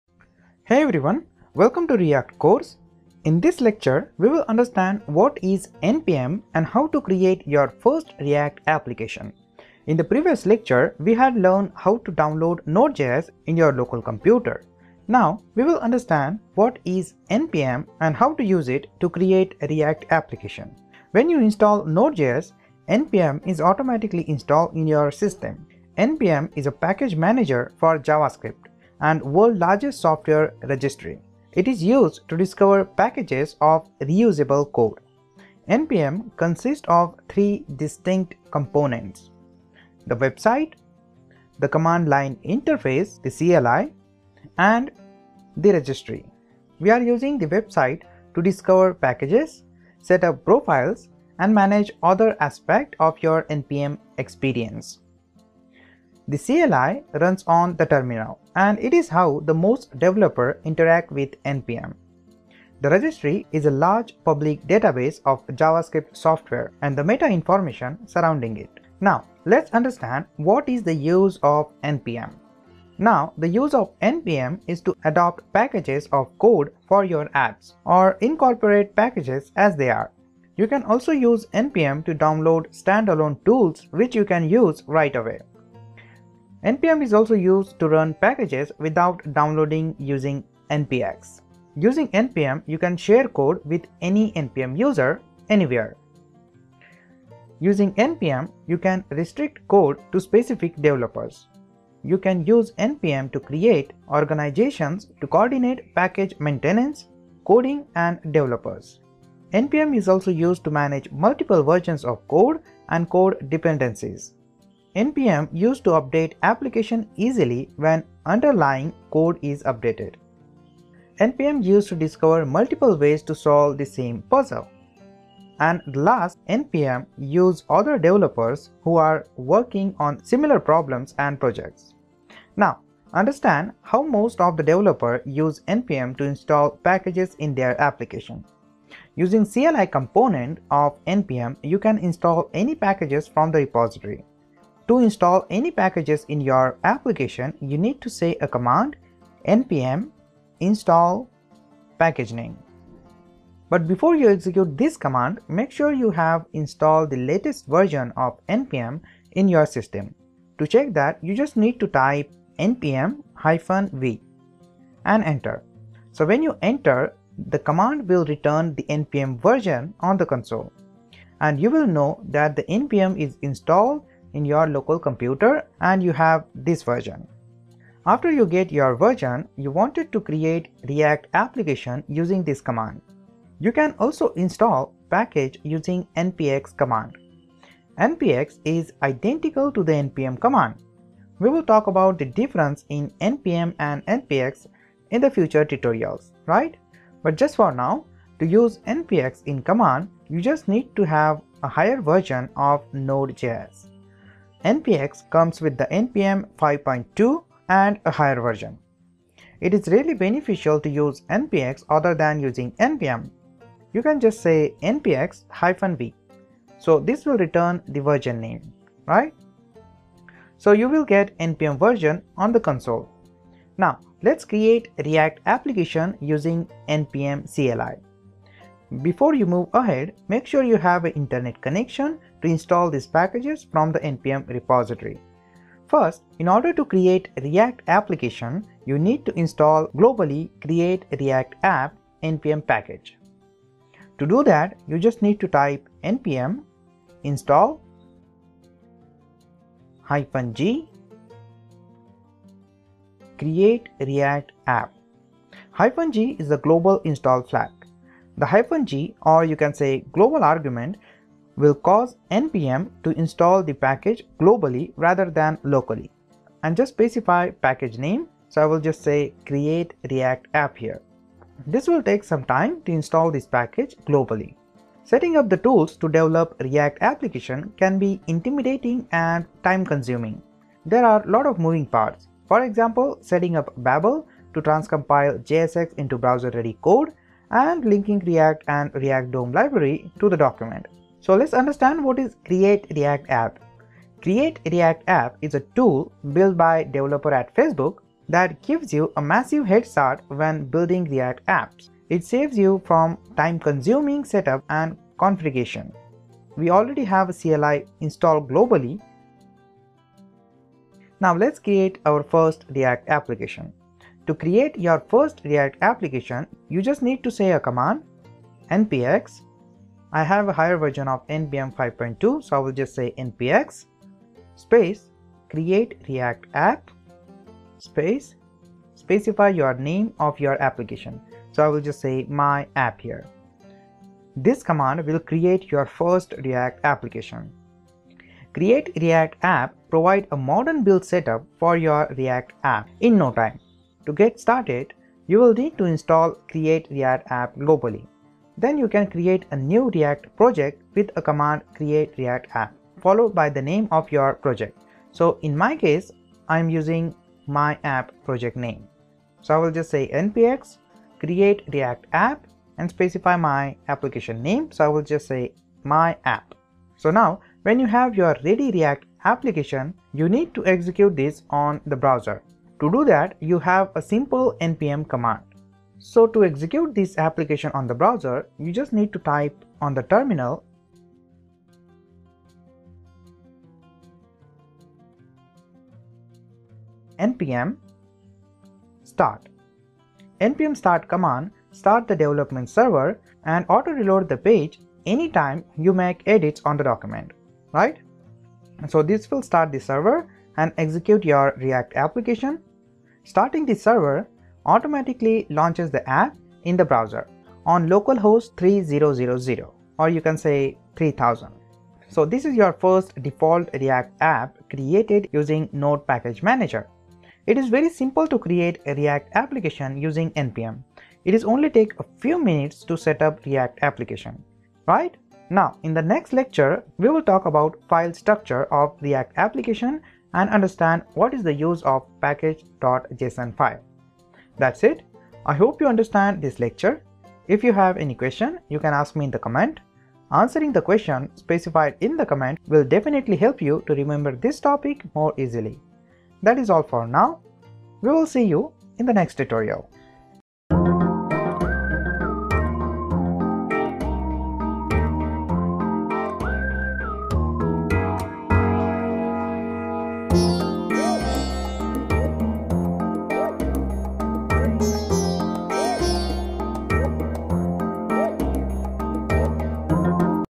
Create First React App Using Node Package Manager ( Npm ) - React For Beginners [18]
Create Your First React App Using Node Package Manager ( Npm ) - React For Beginners [18]
In this lecture we will understand what is npm and how to create your first react application. in the previous lecture we had learned how to download node JS in your local computer.  now we will understand what is npm and how to use it to create react application.
When you install node JS npm is automatically install in your system.
npm is the package manager for JavaScript and the world’s largest software registry. It is use to Discover packages of reusable code.
npm consists of three distinct components:
the Command Line Interface (CLI)
the registry
Use the website to discover packages, set up profiles, and manage other aspects of your npm experience.
The CLI runs from a terminal, and is how most developers interact with npm.
The registry is a large public database of JavaScript software and the meta-information surrounding it.

Now let’s understand what is the use of npm.

Adapt packages of code for your apps, or incorporate packages as they are.
Download standalone tools you can use right away.
Run packages without downloading using npx.
Share code with any npm user, anywhere.
Restrict code to specific developers.
Create Orgs (organizations) to coordinate package maintenance, coding, and developers.
Manage multiple versions of code and code dependencies.
Update applications easily when underlying code is updated.
Discover multiple ways to solve the same puzzle.
Find other developers who are working on similar problems and projects.

Not understand how most of the developer use npm to install packages in their application.
using cli component of npm you can install any packages from the repository.

To install any packages in your application you need to say a command.
Npm install packageName

But before you execute this command make sure you install the latest version of npm in your system to check that you just need to type.
Npm -v
this command will return the npm version on the console And you know that node JS is installed in your computer.

 after you get your version you wanted to create react application using this command.

You can also install package using npx command.
 npx is identical to the npm command.
 we will talk about the difference in npm and npx in the future tutorials.
 to use npx in the command you need to have a higher version of node JS.
npx comes with npm 5.2+ and higher

It is really beneficial to use npx other than using npm.
 so to check npx is working or not  I will just say.
Npx -v

so you will get npm version on the console right.

Now let's create react application using npm cli
Make sure you have internet connection to install this packages from the npm repository.
First in order to create react application you need to install globally create react app npm package.

npm install -g create-react-app

-g is the global install flag,The -g or --global argument will cause npm to install the package globally rather than locally.

And just specify the package name so I will say create react app here.
And press enter.

This will take some time to install this package globally.

Setting up the tools to develop a React application can be intimidating and time-consuming. There are a lot of moving parts. For example, setting up Babel to transpile JSX into browser-ready code and linking react and reactdom library to the document.
Create React App is a tool (built by developers at Facebook) that gives you a massive head start when building React apps. It saves you from time-consuming setup and configuration.
Now that we have the CLI installed globally, we can create our first React application:

To create your first react application you just need to say command

npx create-react-app my-app

this command will create your first react application.
Create React App provides a modern build setup for your React apps in no time. To get started, you’ll need to install Create React App globally.
You can create a new React project with the command create-react-app, followed by the name of your project. In my case we used  npx create-react-app my-app.

Now when you have ready react application is ready you need to execute this on the browser to do that you have a simple npm command.

Npm start

npm start starts the development server and auto-reloads the page any time you make edits

Starting the server (npm start) automatically launches the app in the browser on localhost:3000.
So this is your default react app created using the npm.